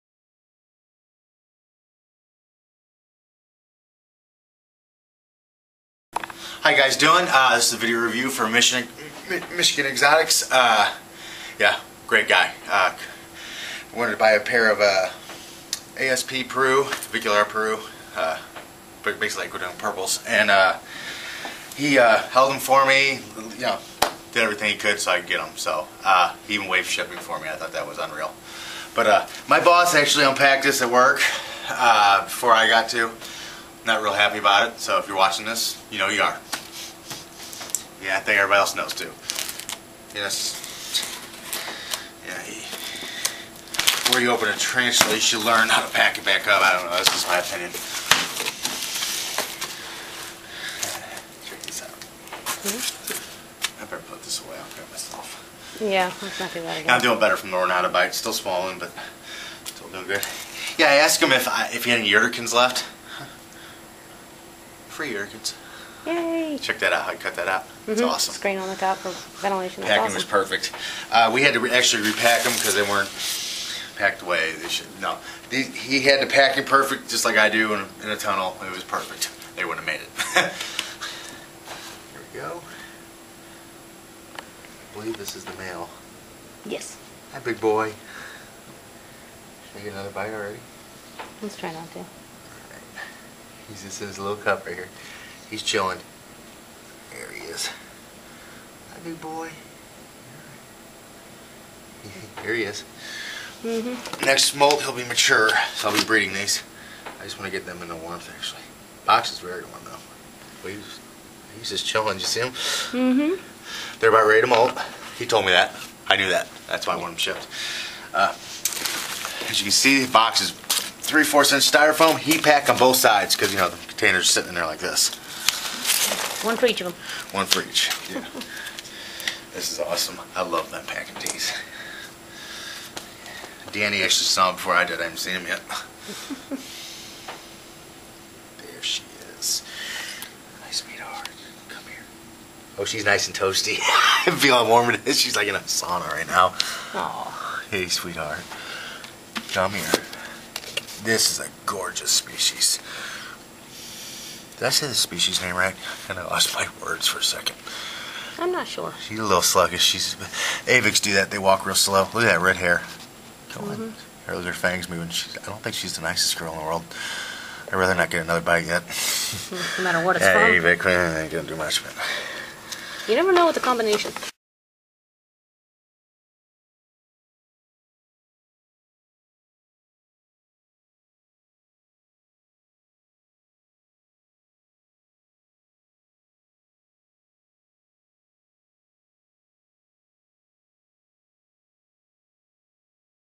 Michigan Exotics review!!!
I recieved a pair of A. Sp "Peru" from Michigan exotics and he came thru with flying colors!!!